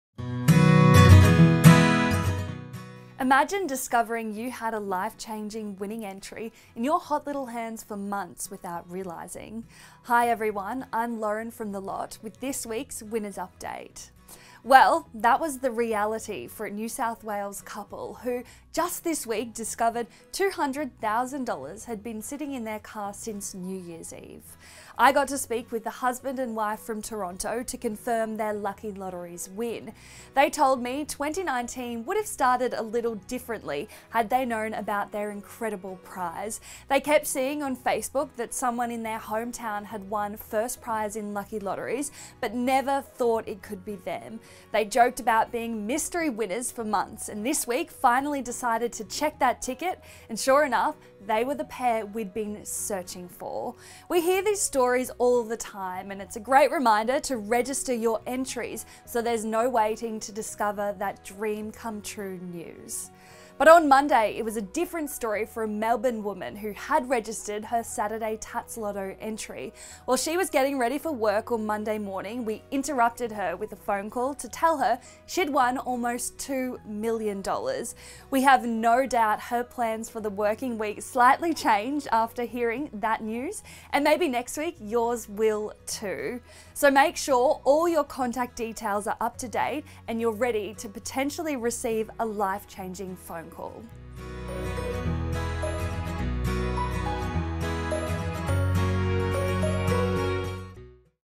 Weekly Winners Wrap | 8th Mar 2019 | the Lott Australia's Official Lotteries
The Lott's weekly winners wrap for 8th March 2019. Hear real winners’ stories across all of our games including Powerball, Oz Lotto, Instant Scratch-Its and more!